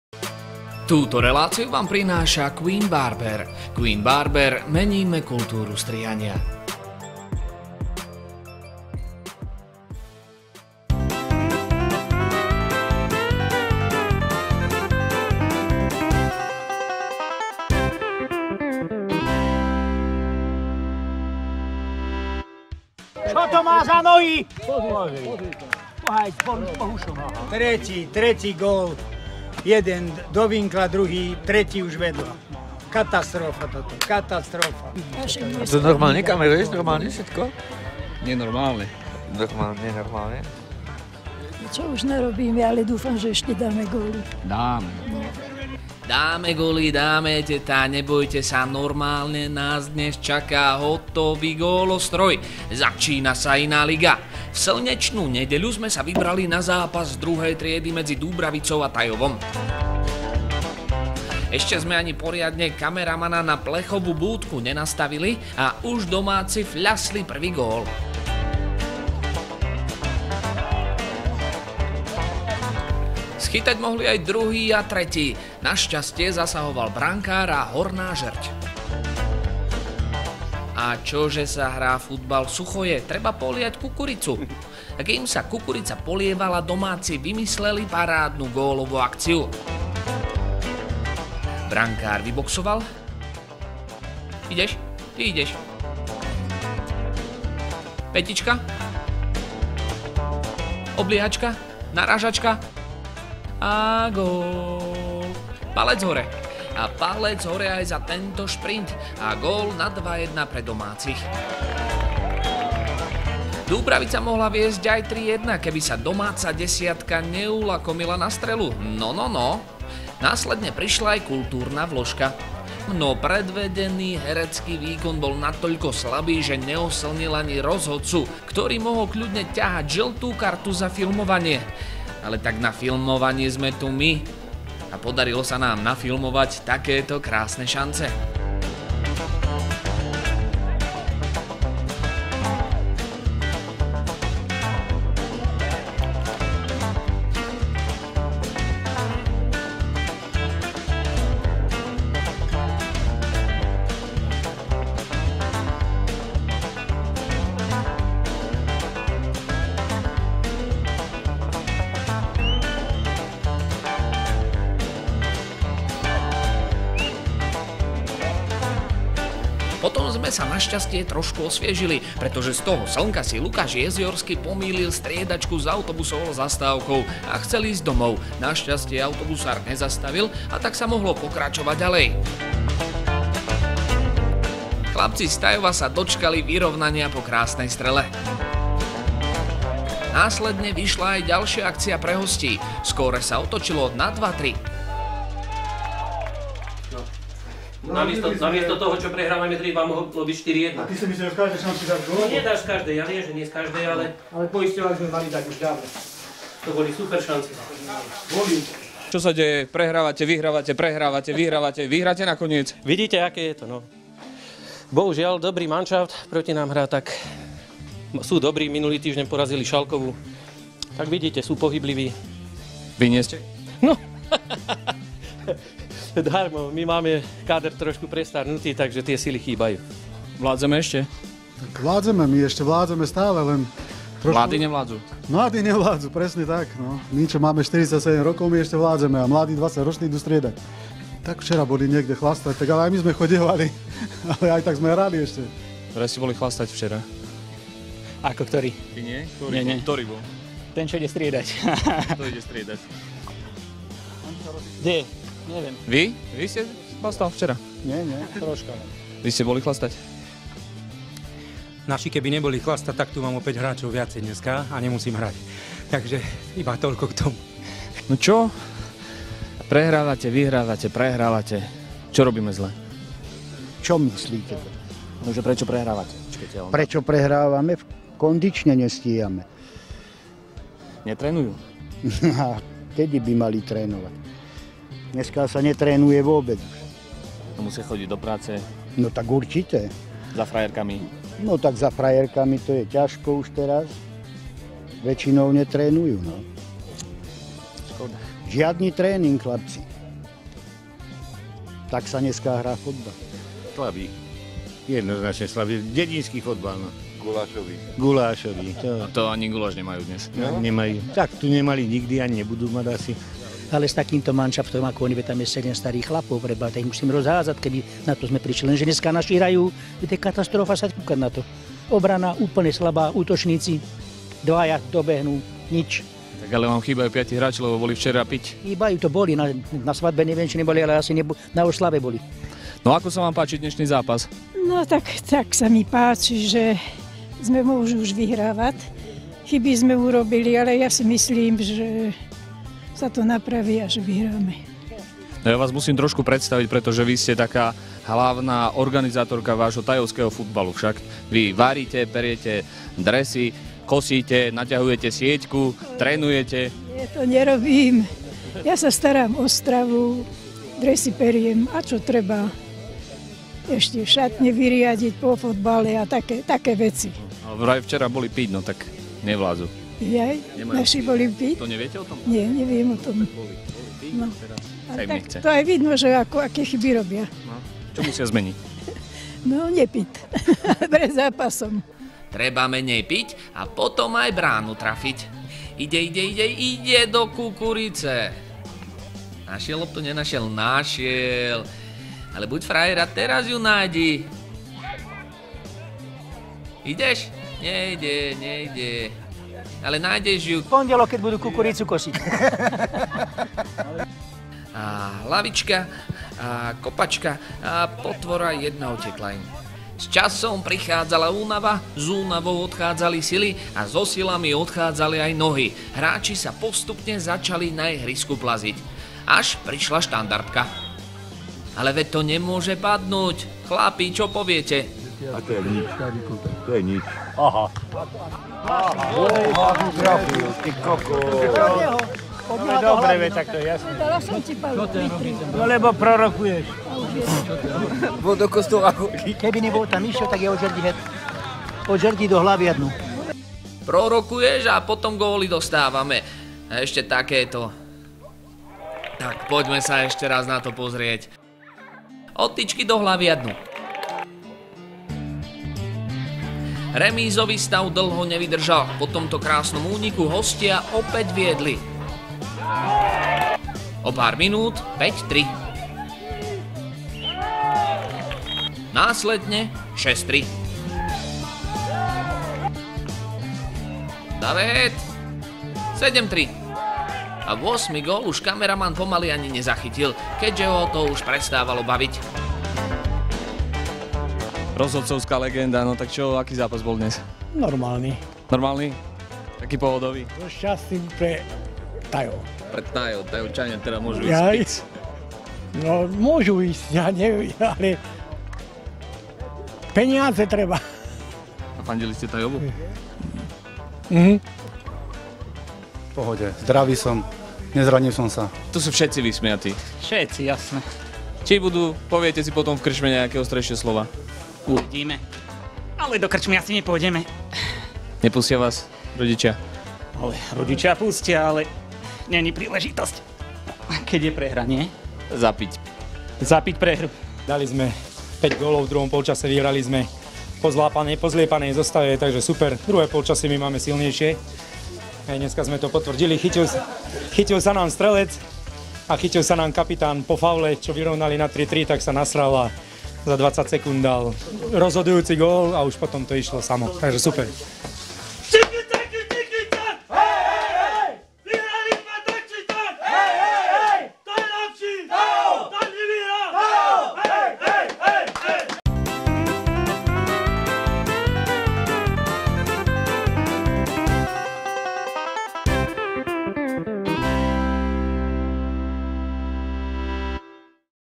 Dúbravica 🆚 Tajov │ INÁ LIGA
🏆🏆11 gólov, brankár v kukurici 🌽🌽 fanúšikovia futbaloví experti 🍺🍺 a kameraman na streche 😂😂 Keď ide o dobré zábery, riskujeme aj pády. Tomu sa povie Iná liga  😍😍👍💪
TJ Družstevník Dúbravica 🆚 TJ Slovan Tajov (7. liga │ ObFZ Banská Bystrica)

... a neujde Ti žiadne nové video.

... a podpor tak INÚ LIGU.

... a sleduj, čo chystáme najbližšie. 👀